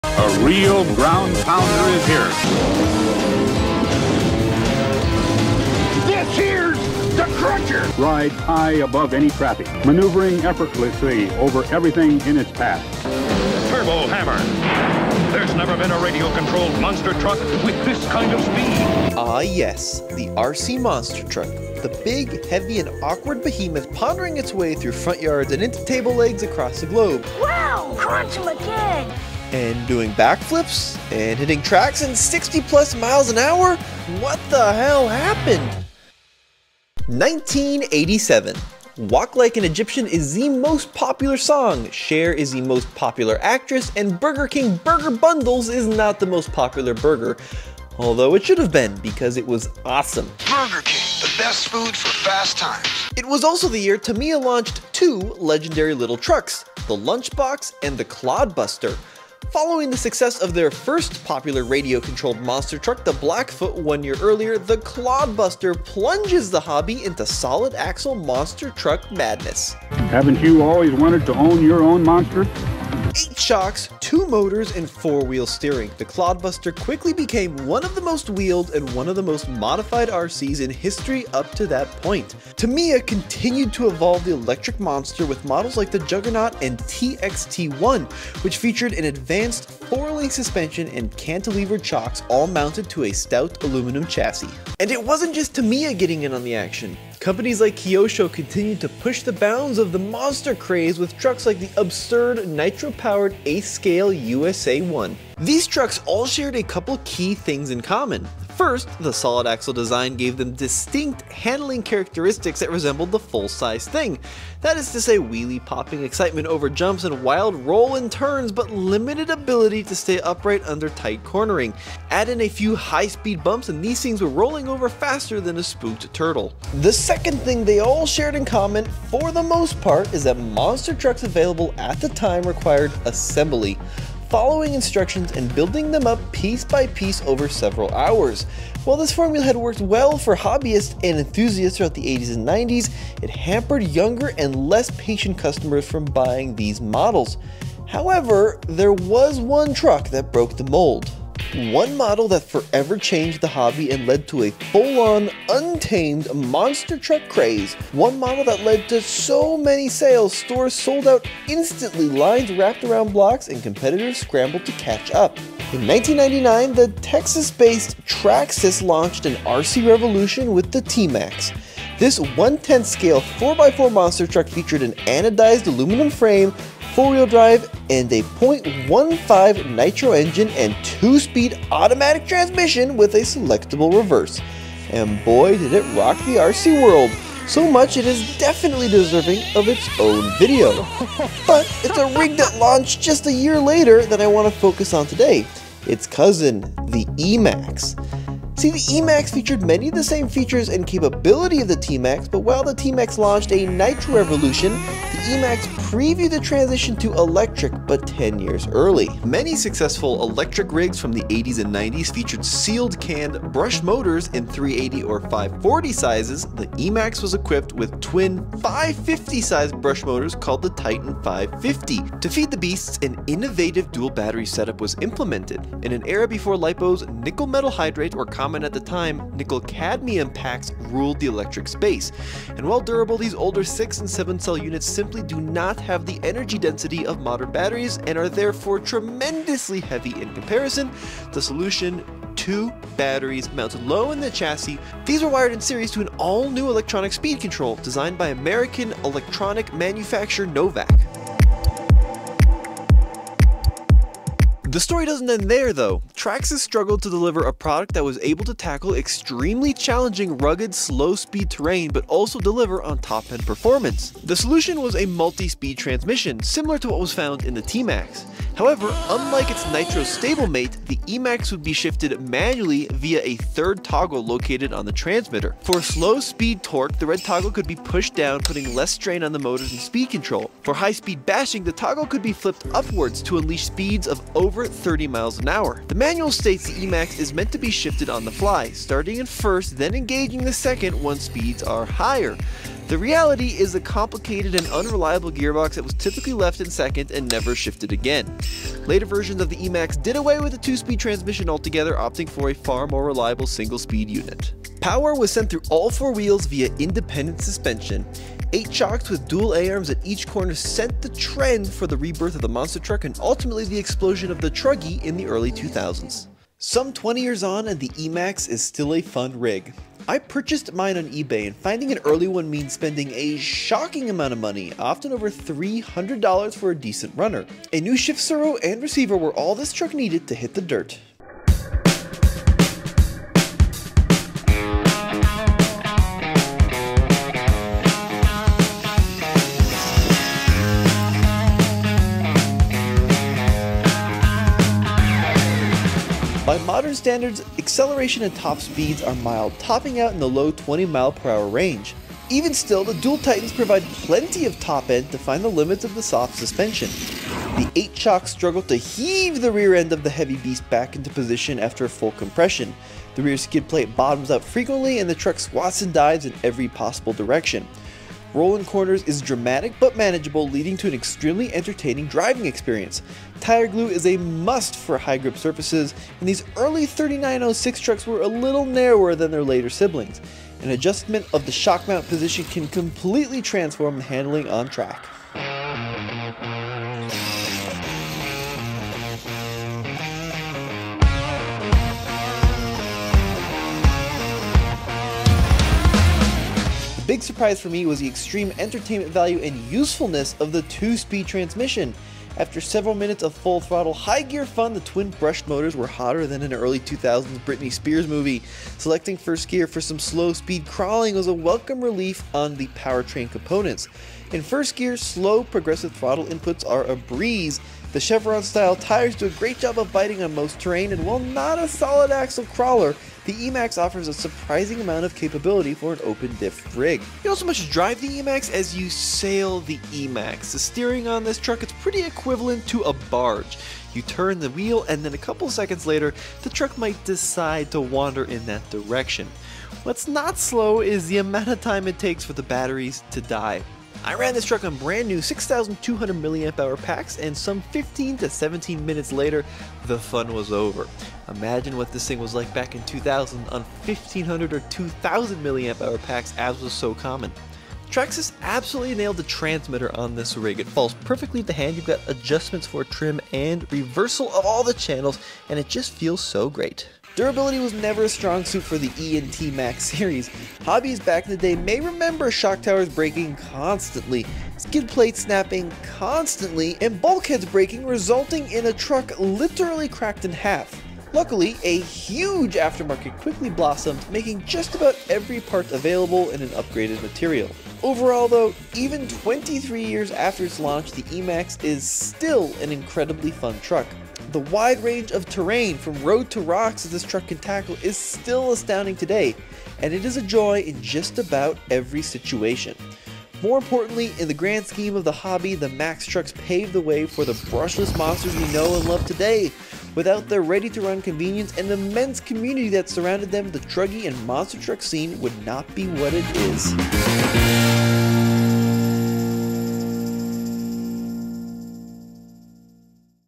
The Truck That Changed The World (And Broke a Lot) - Traxxas E-Maxx Story
The Traxxas T and E-Maxx are two 1/10th scale monster trucks that forever changed the radio control landscape. Launched in the late 1990's these two models were a game changer in the RC landscape and will forever be remembered for their performance and fun factor.